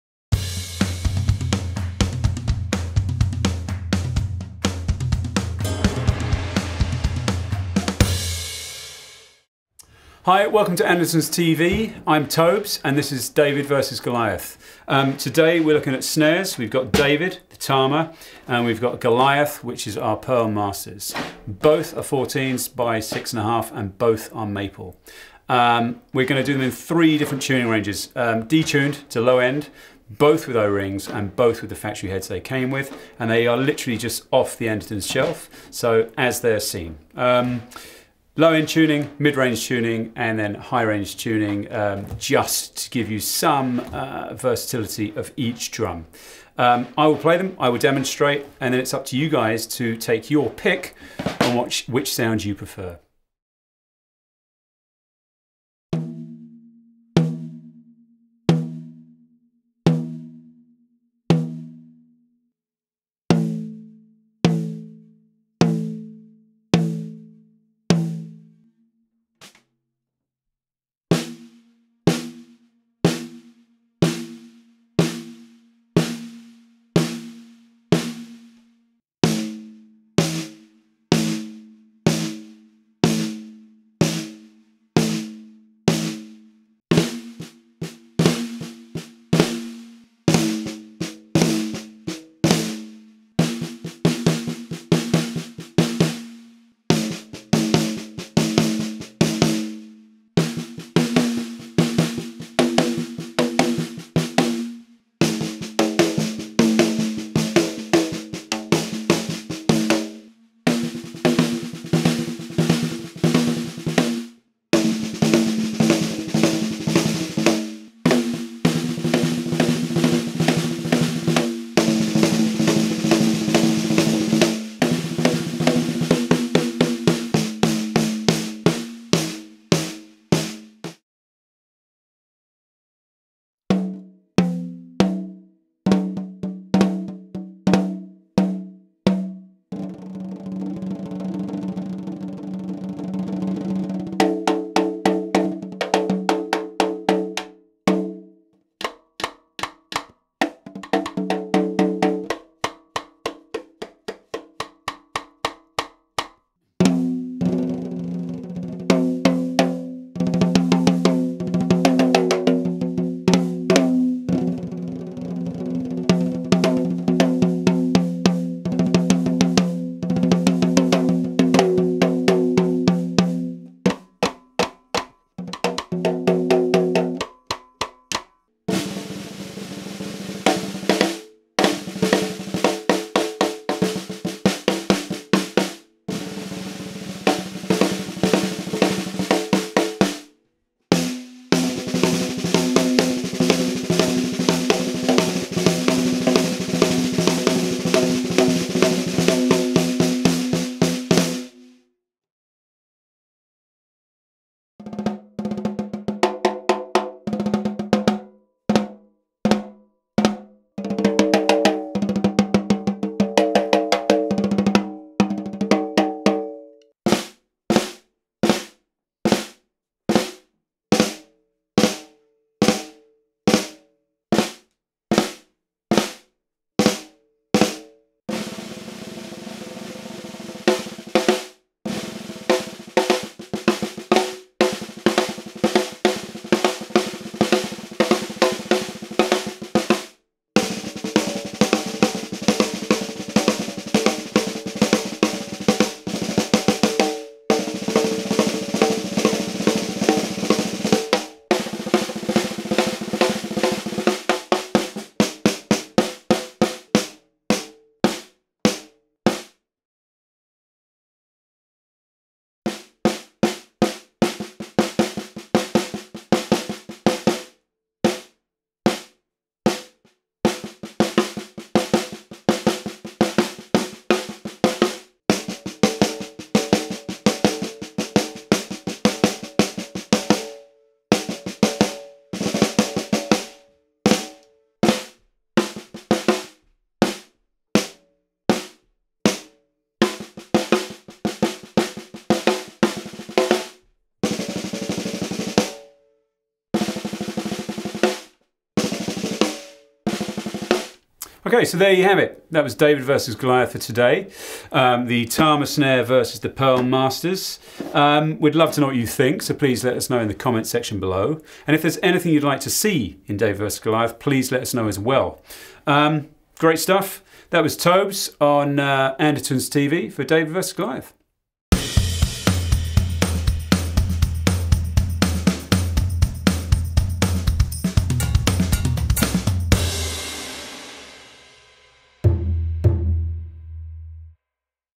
Drum Addicts - Snare Vs. Snare - Pearl Maple 1-piece & Tama Maple
👆 Drum Addicts - Snare Vs. Snare - Pearl Limited Edition 1-Piece Maple 14" goes up against a Tama 14" Maple.

Watch Tobes demonstrate the difference between one high-end Pearl snare against a cheaper Tama option.

We've done this in three different tuning ranges with the same drum heads and microphones to help you choose your favourite snare.